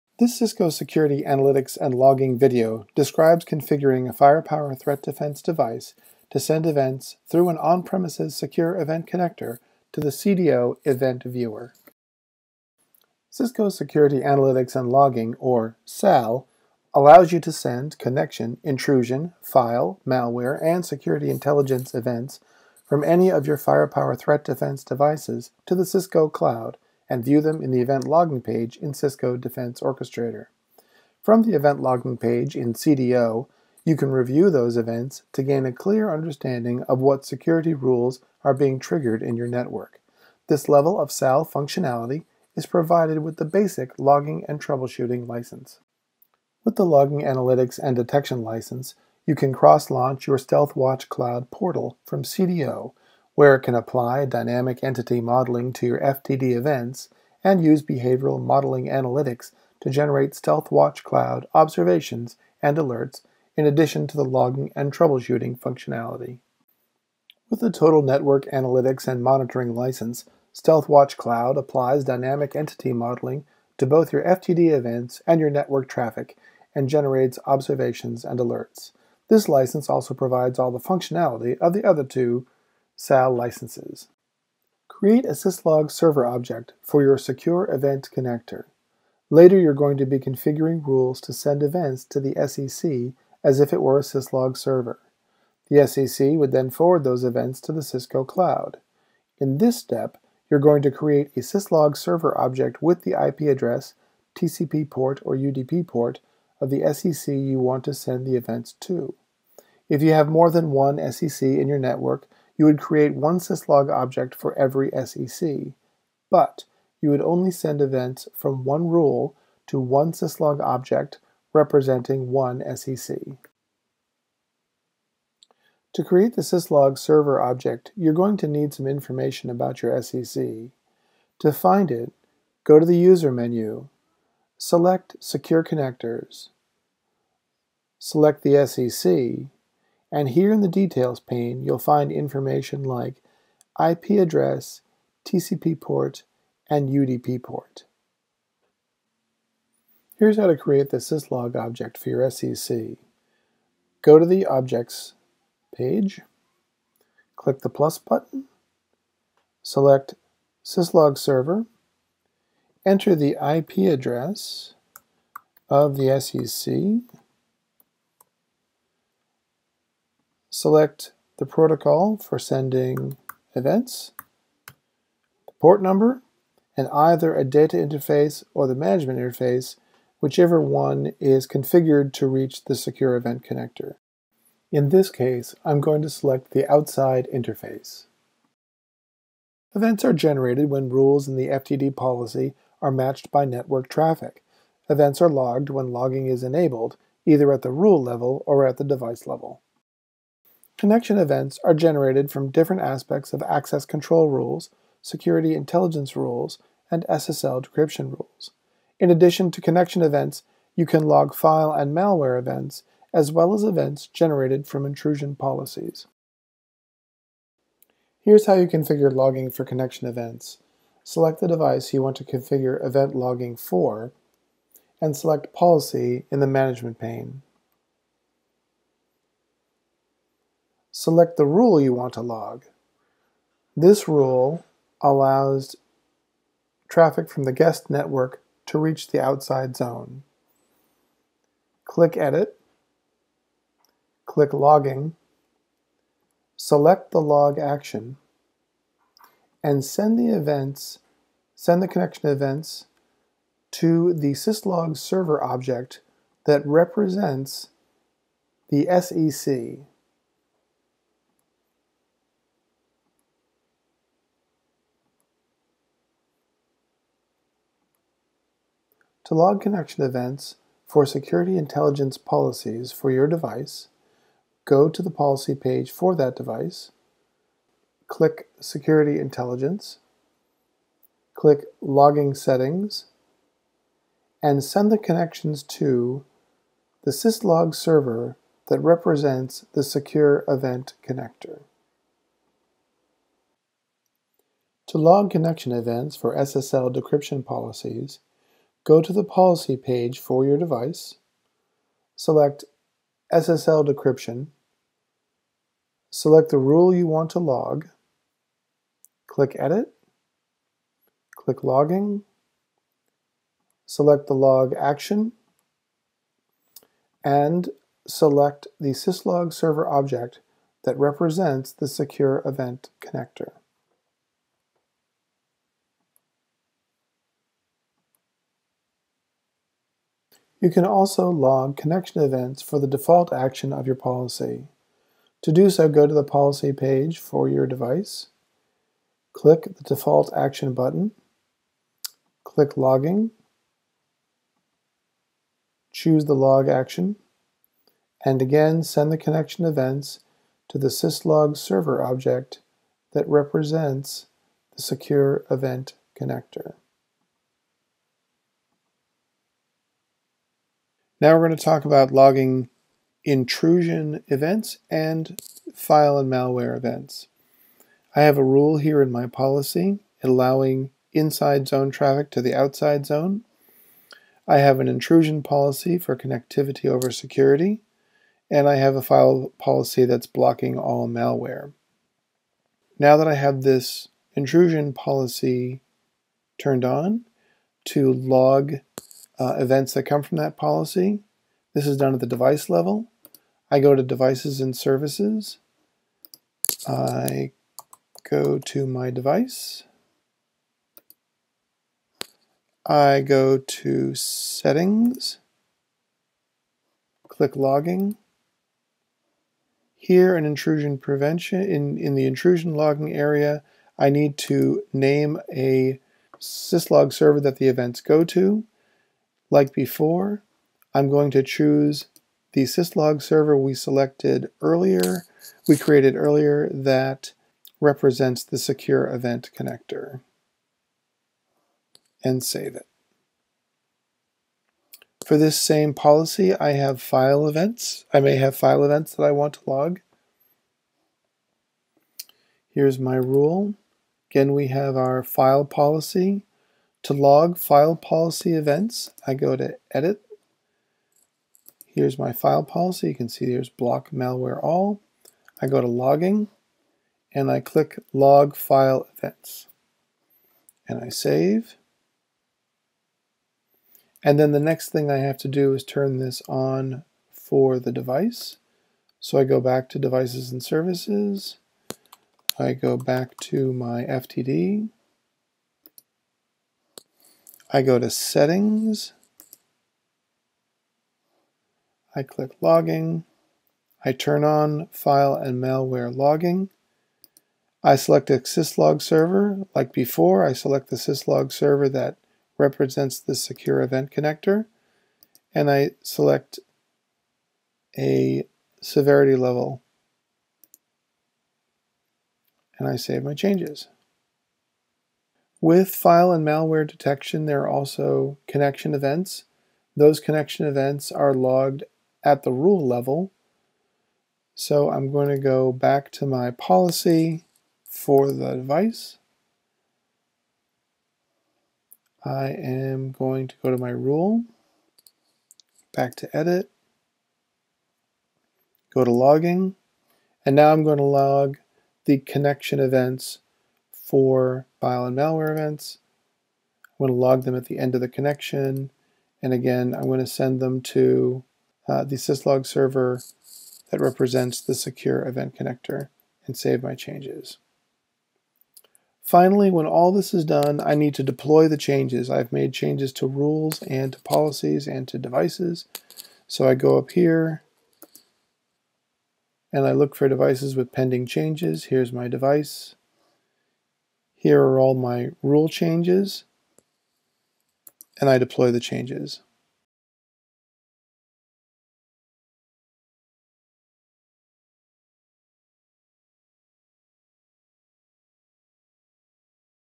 Security Analytics and Logging SaaS Event Viewer Configuration
This video provides a technical demonstration of how to send Firepower Threat Defense (FTD) events to Security Analytics and Logging for scalable, real-time logging in the cloud.

Timestamps:
0:00 - Overview of Security Analytics and Logging
1:25 - Create a syslog server object for your SEC
3:11 - Configuring FTDs to log events